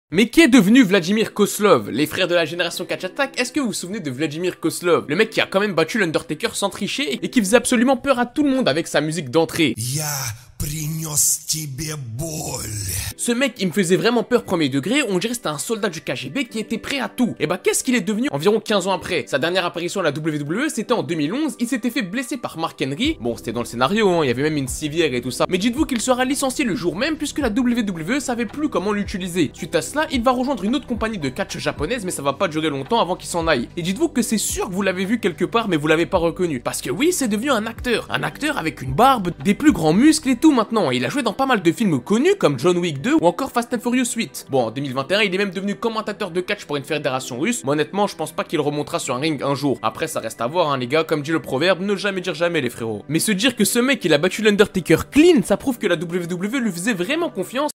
Mais qu'est devenu VLADIMIR KOZLOV ? 🇷🇺
Figure de la génération catch attack pendant quelque mois, le catcheur Vladimir Kozlov est tombé dans l'oublie ! Je t'explique tout ça dans ce short consacré au Catch et à la WWE ! 😎